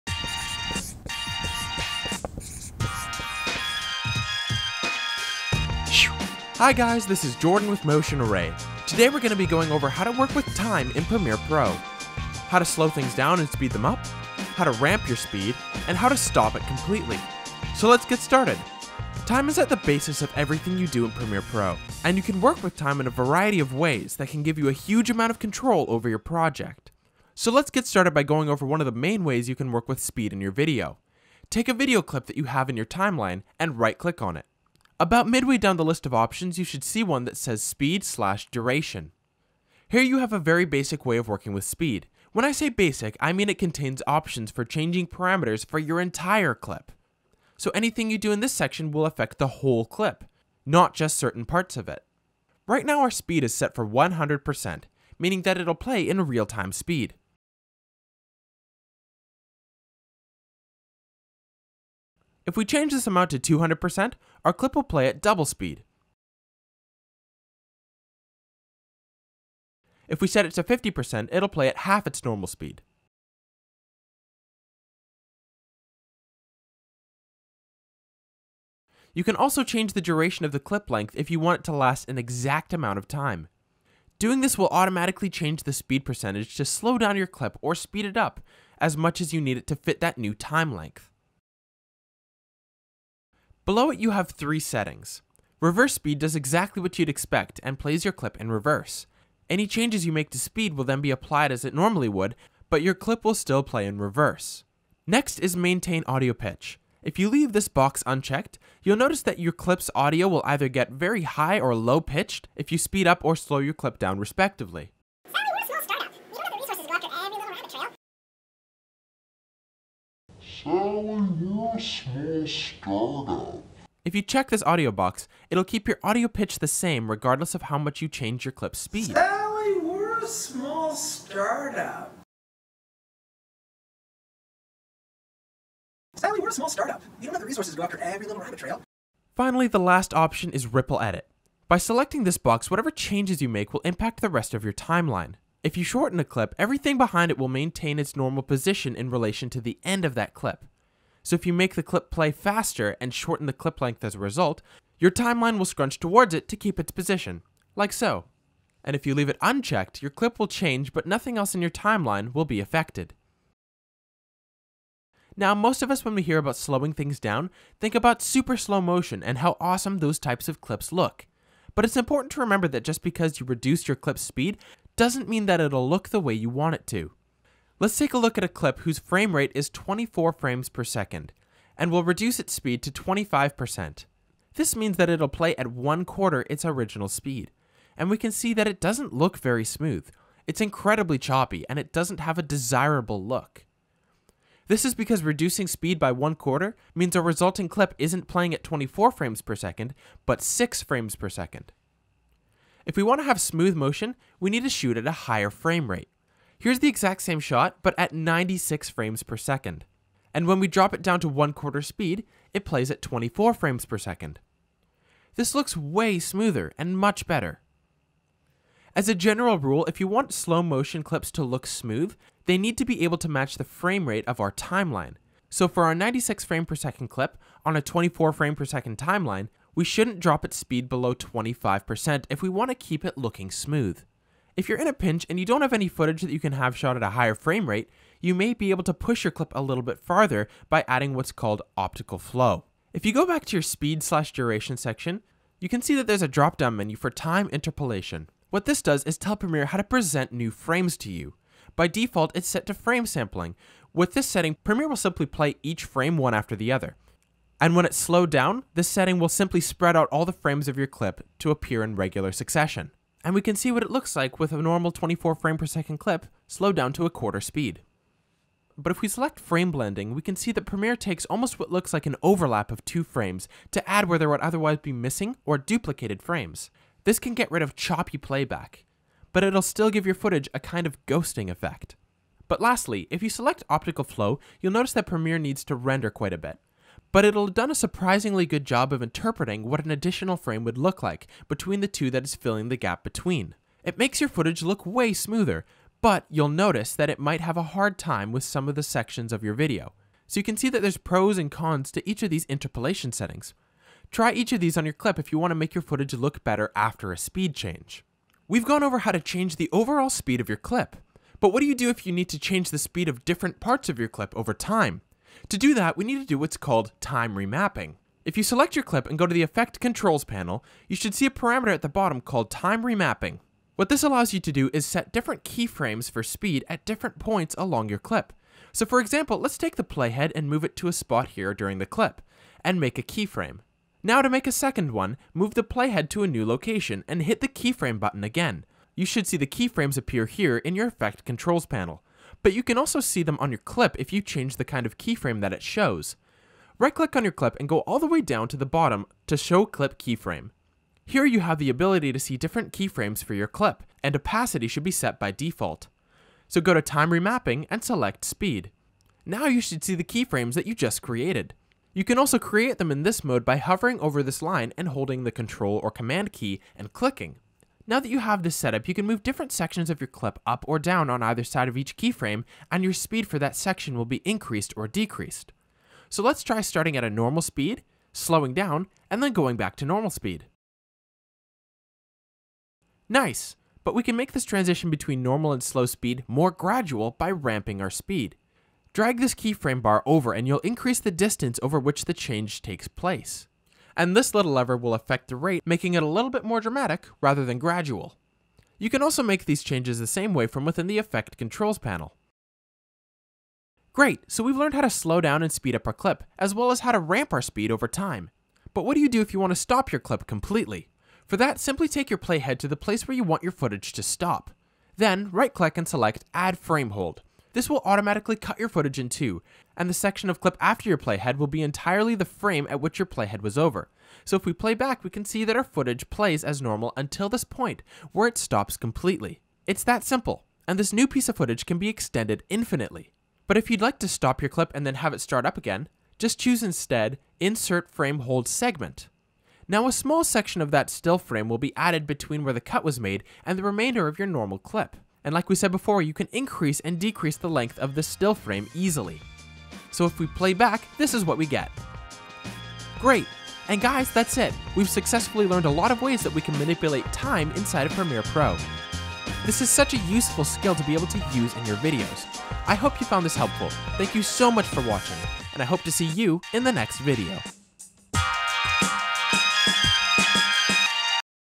How To Get Slow Motion, Speed Ramping, and Frame Holding in Premiere Pro - Premiere Pro Tutorial
Check out our full article on slow motion in Premiere Pro!

Slow Motion, Speed Raming, and Frame Holding in Premiere Pro! These are all really desirable traits to have in your premiere pro video! In this video we explore all about how to work with speed in Premiere Pro.  Time is at the core of how video editing works, so understanding how to use it to your advantage is can give you amazing control over your project.  There are a variety of ways to work with speed in Premiere Pro, but three of the most essential are the basic speed duration editor, time remapping, and stopping time in your video.